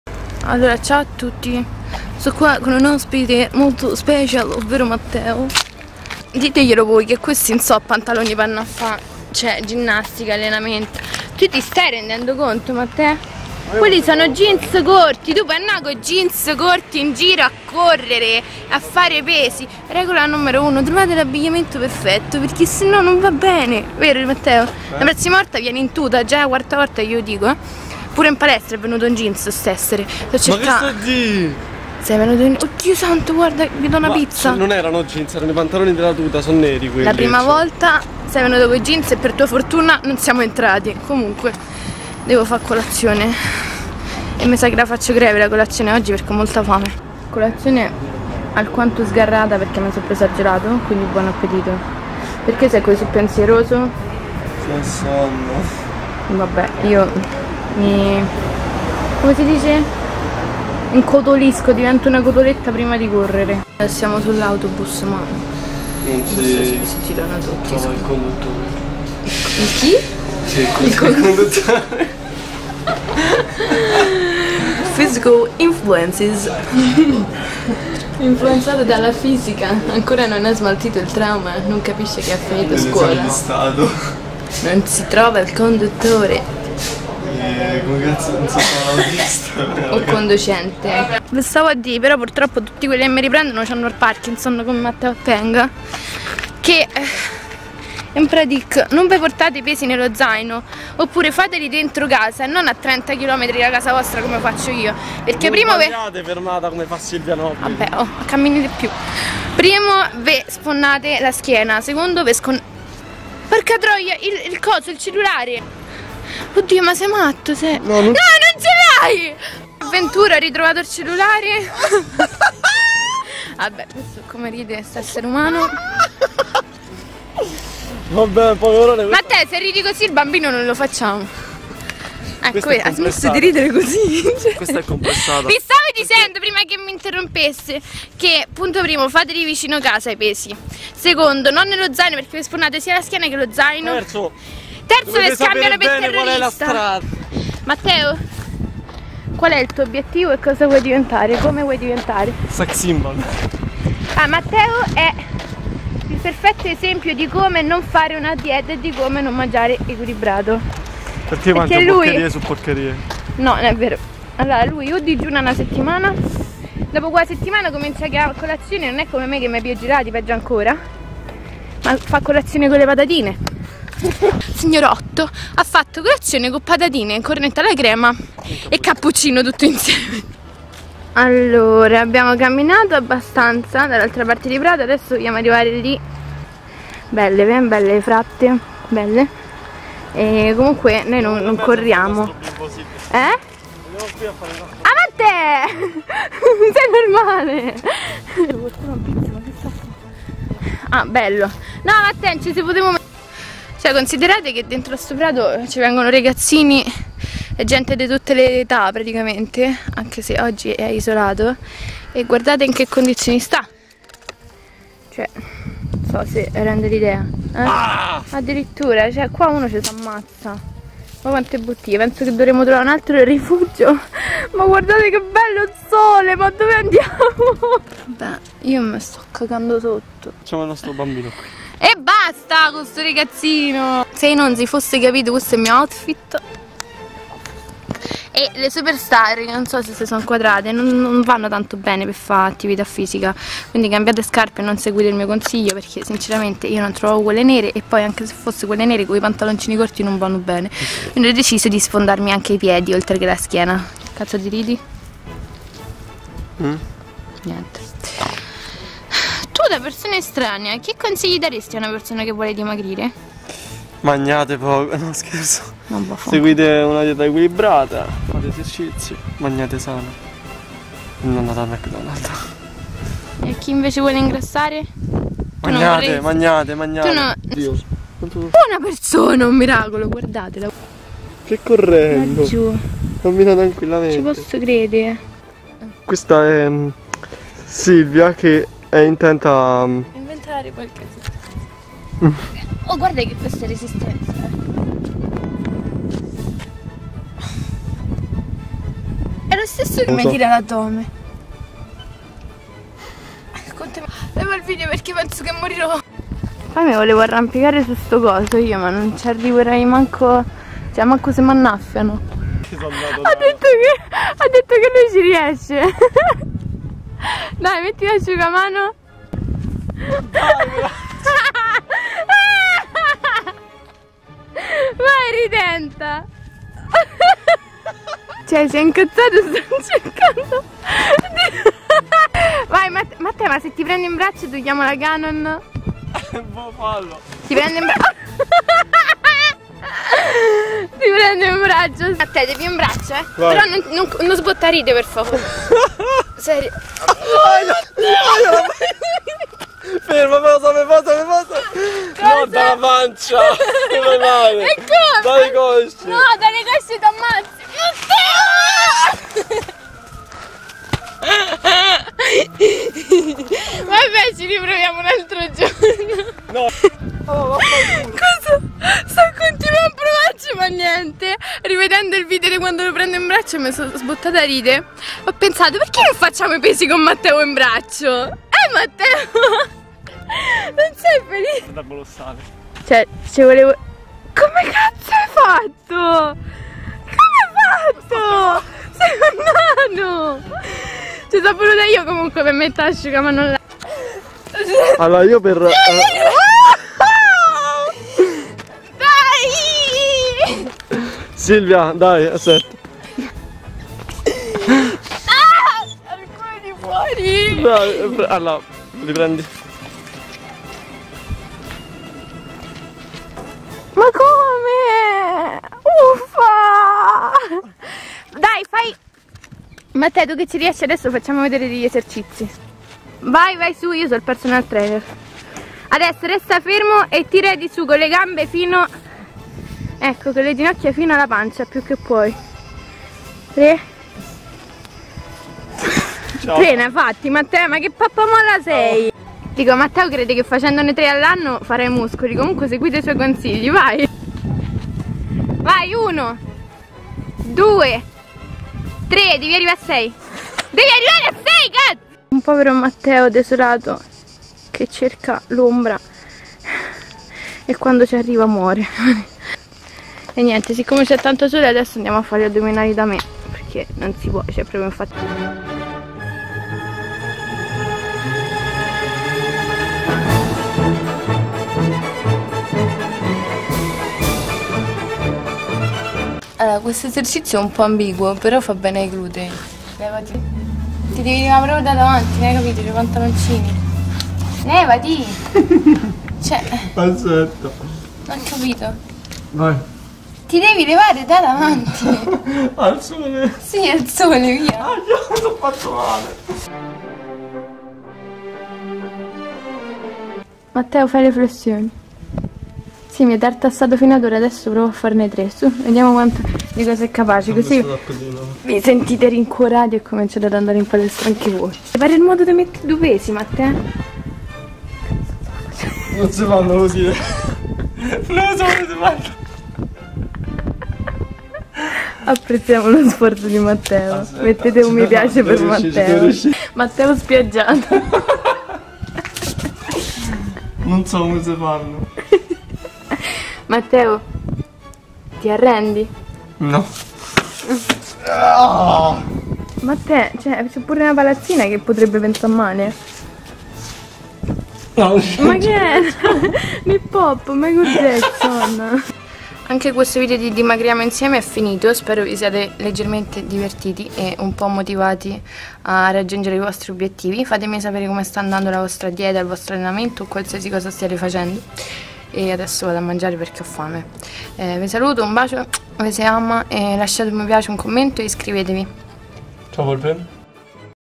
VOLETE DIVENTARE PALESTRATI? DIMAGRIAMO INSIEME - EPISODIO 2 | Polpetta.
In questi miei vlog voglio farvi capire che con un po' di pazienza e forza di volontà, tutti raggiungeremo i nostri obiettivi.

Periscope: @silvianobili_